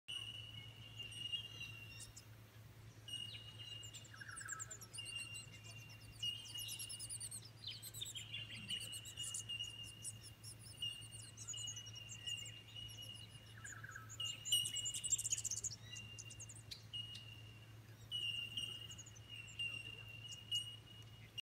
Superb Blue Wrens frolicking at Ada River
Bell Birds chimed from the Mountain Ash all through the day. Superb Blue Wrens flitted amongst the ferns. Errinundra National Park is a very special place.